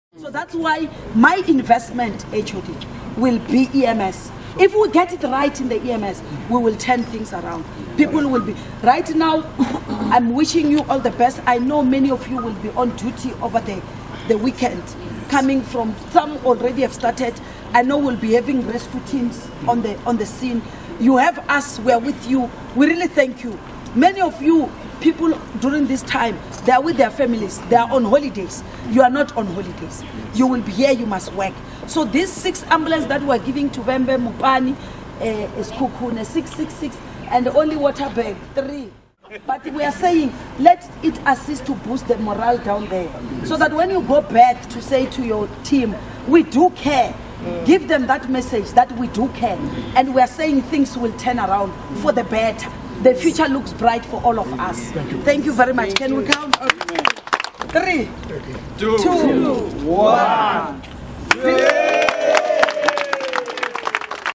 Lim MEC for Health on EMS personnel
During the handover of 36 ambulances and medical vehicles by MEC Phophi Ramathuba, she emphasized the importance of emergency medical service (EMS) personnel.